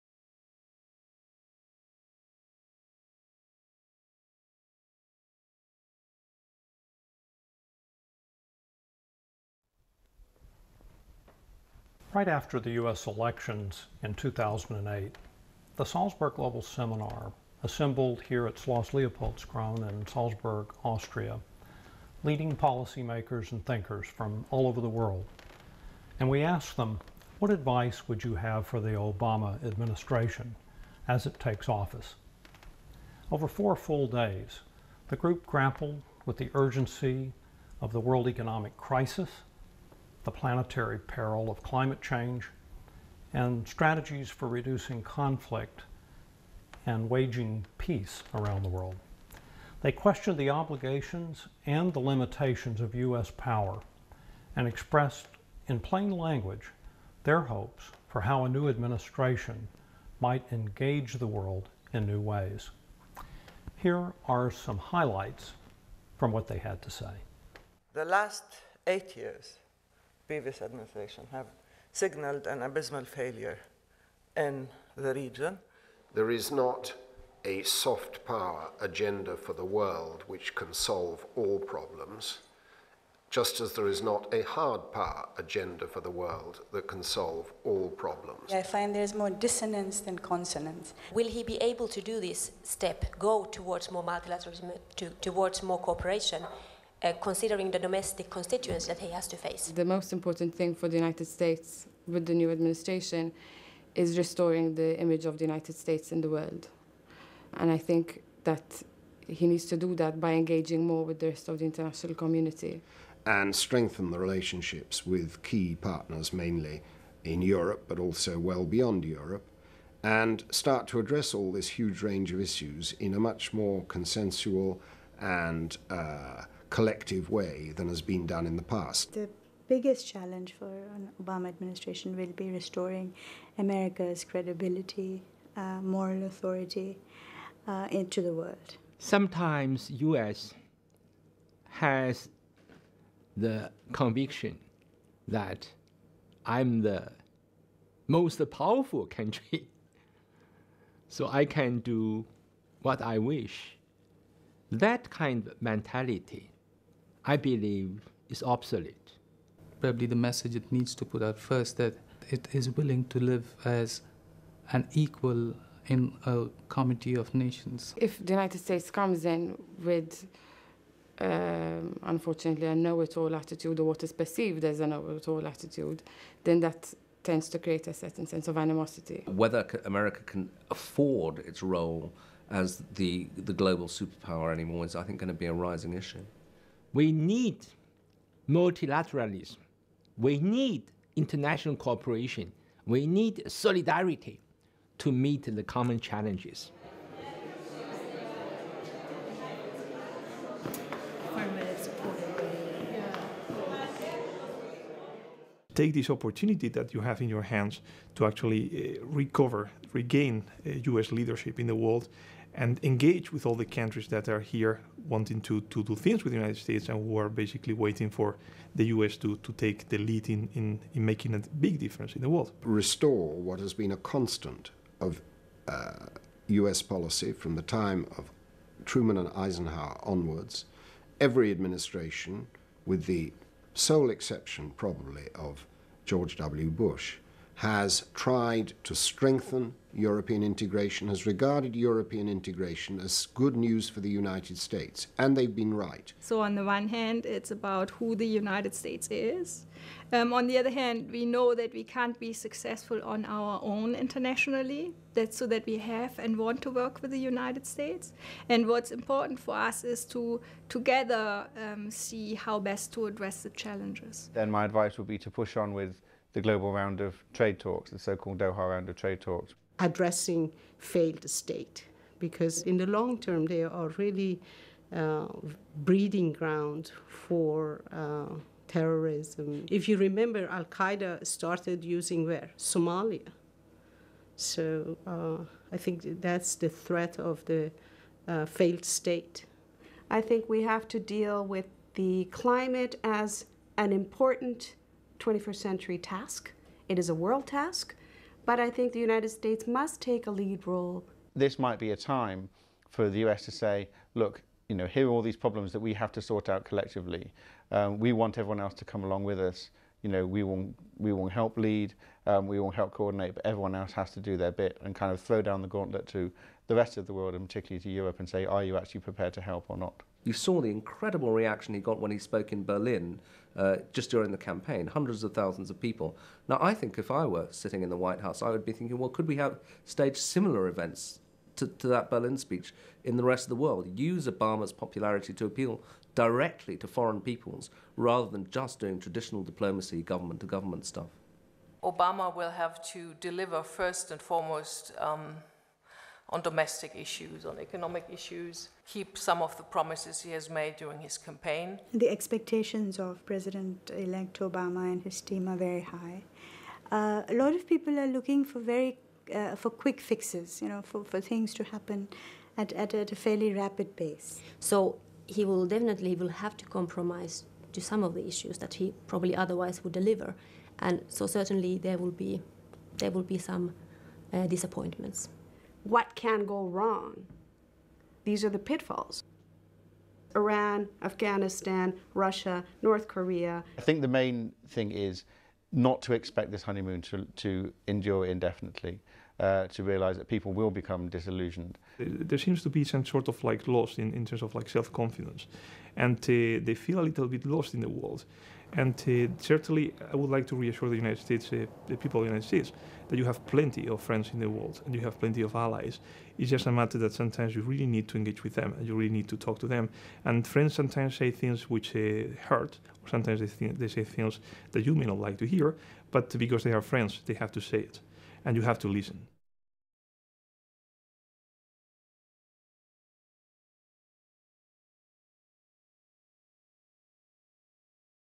Roadmap for Re-engagement: The World's Advice to the New US Administration
From November 10-14, 2008, 44 decision makers and analysts from 24 countries outside the United States (24 Europeans, 17 Asians, 3 from the Americas and Australasia) met with 13 of their US counterparts in Salzburg, Austria, from November 10 to 14, 2008, to discuss The United States in the World: New Strategies of Engagement. The resulting 4 days of dialogue and debate confirmed that the new administrations long-term objectives are peace, stability and prosperity both at home and abroad, and that the avowed aim of its foreign policy should be to build and maintain a world economic and political order based on mutual openness, international cooperation, and observance of agreed rules. This video serves as a record, in part, of that meeting, and of the views of the assembled experts.